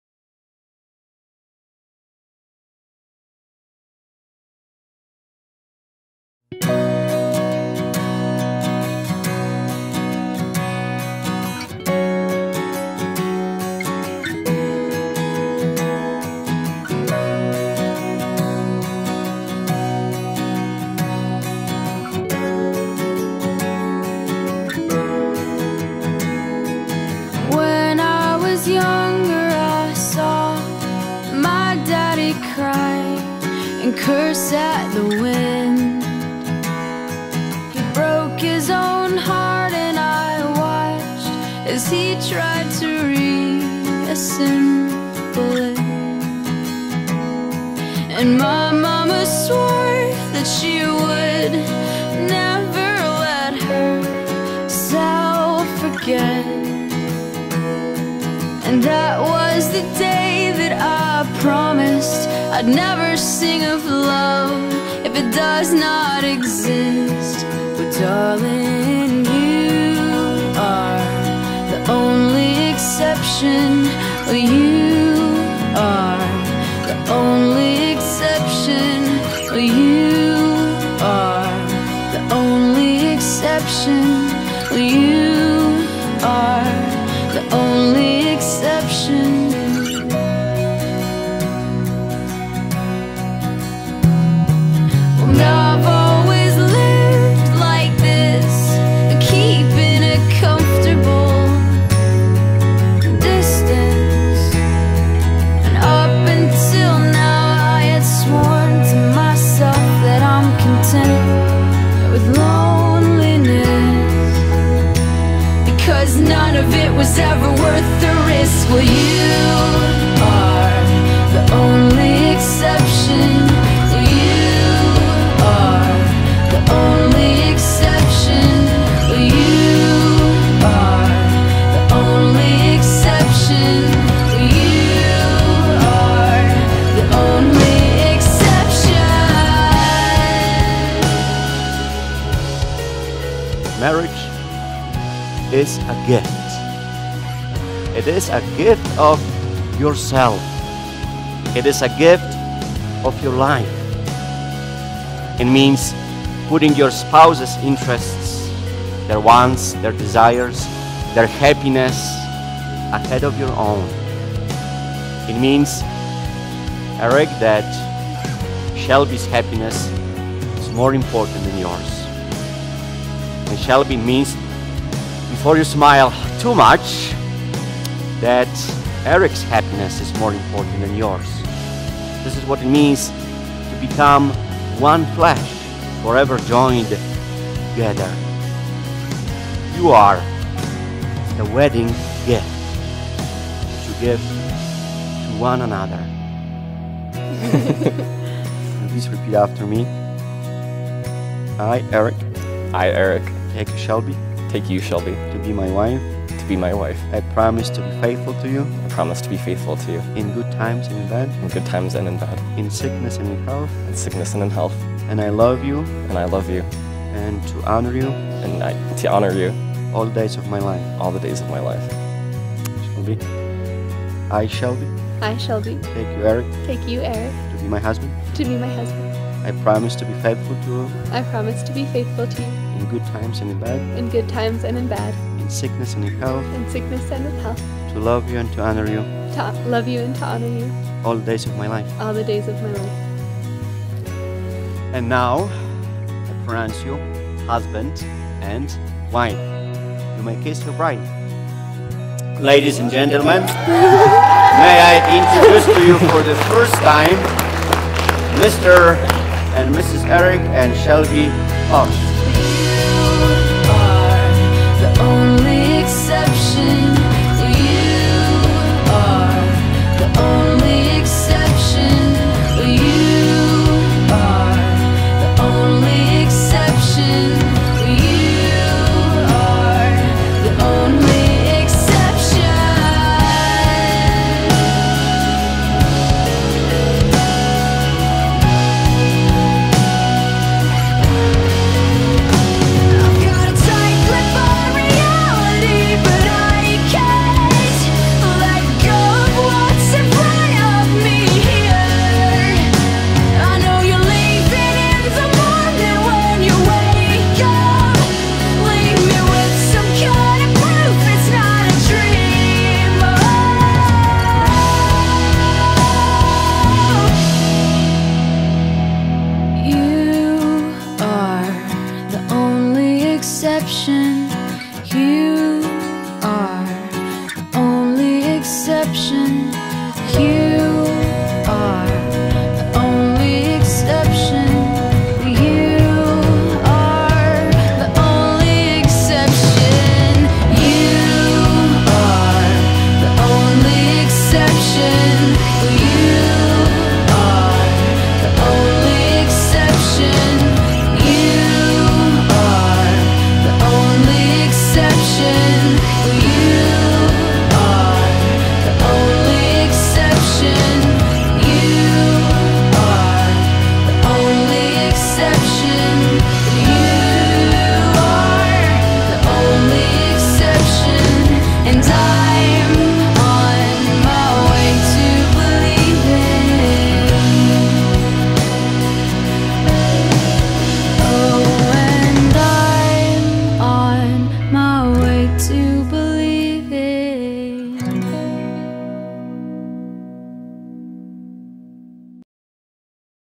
A Wedding Highlight Video at Our Lady of the Pines Catholic Church and The Warehouse
Just a quaint little wedding at a Catholic church in Black Forest... not! Shelby and Eric's epic love story culminated in a stunning wedding at Our Lady of the Pines and a rowdy reception worthy of the history books at The Warehouse Restaurant.